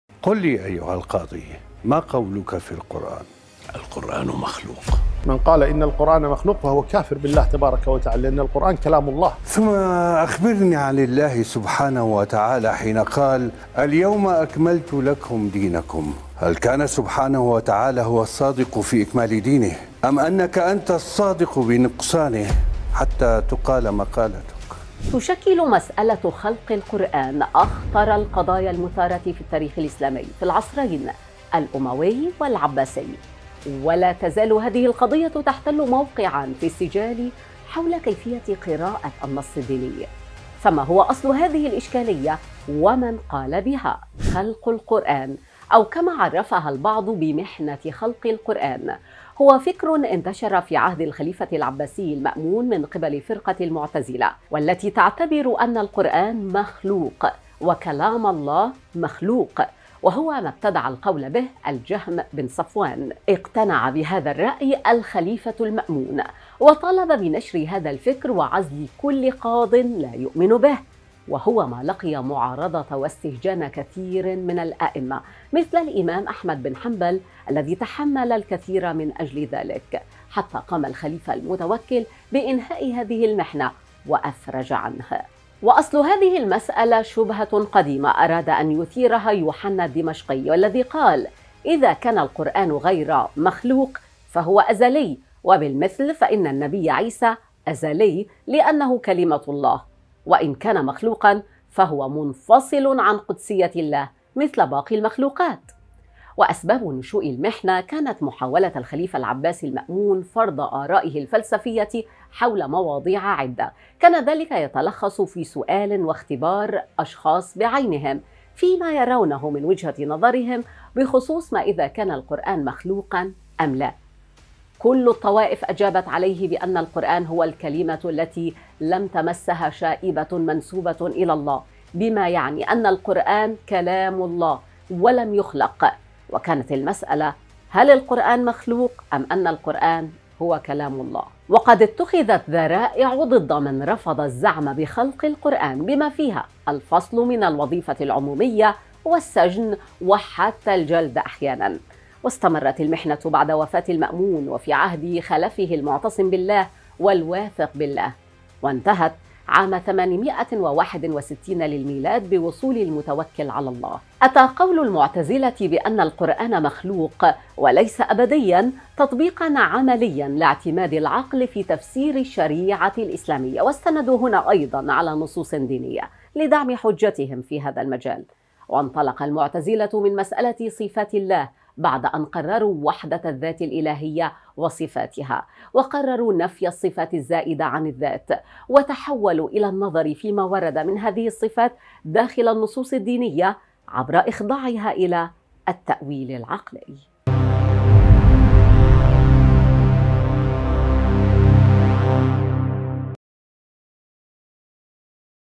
"خلق القرآن" فتنة المعتزلة التي قسمت الأمة وصدقها الخليفة المأمون
تشكل مسألة خلق القرآن أخطر القضايا المثارة في التاريخ الإسلامي في العصرين الأموي والعباسي ولا تزال إهذه القضية تحتل موقعاً في السجال حول كيفية قراءة النص الديني... فما هو أصل هذه الإشكالية ومن قال بها؟